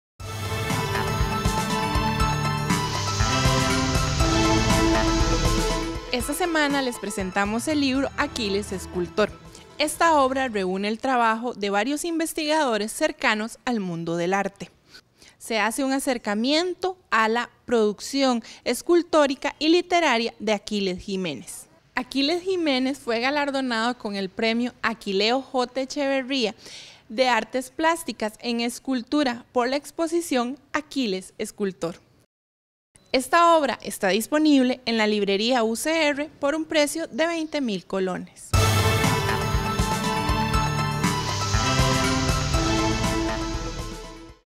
Desde la Editorial 12feb
Le presentamos el libro que nos recomienda esta semana la Editorial de la Universidad de Costa Rica.
Le recordamos que este libro puede ser suyo. Para participar en el sorteo ingrese a la página de facebook del informativo y siga las instrucciones.